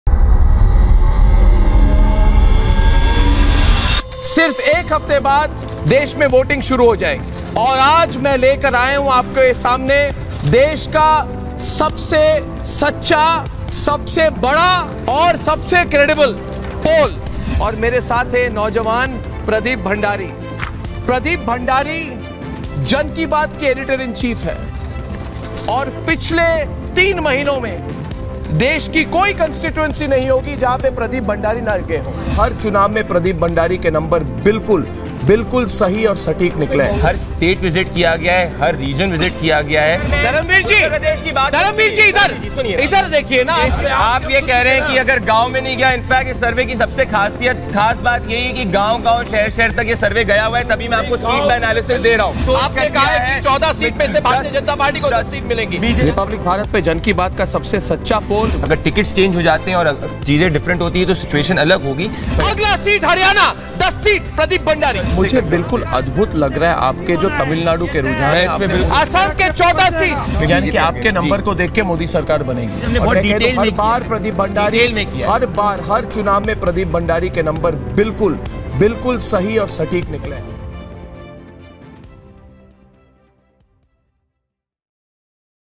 'Pradeep Bhandari,the youngster who has travelled every corner of the country', said Arnab Goswami
Presenting Pradeep Bhandari,the youngster who has travelled every nook and corner of the country with a smashing track record of every election prediction to be 100% accurate, said Arnab Goswami, Managing Director, Republic media network